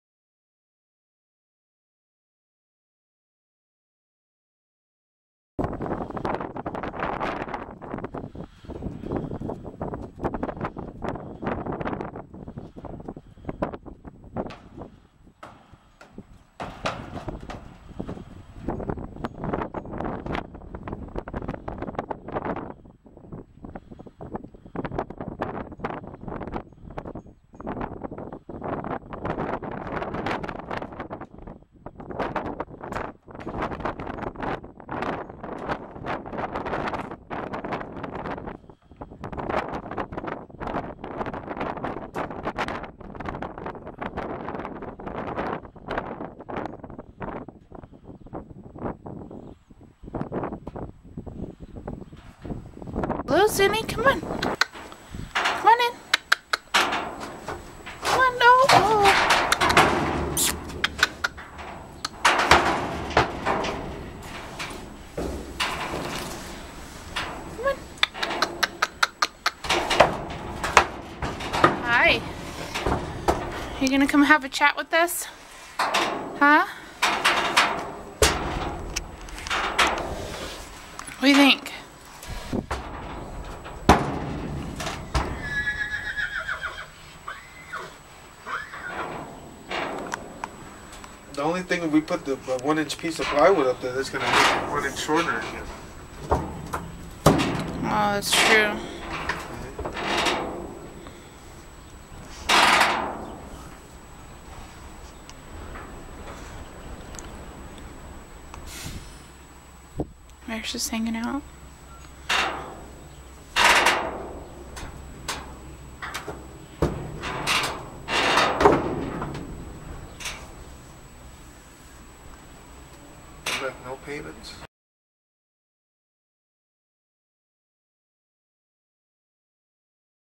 Zuni - Trailer Loading
This is BLM mustang Zuni her third time loading into the trailer. She was very quiet about the whole process and very easy to teach. You can view her blog and our available adoptable mustangs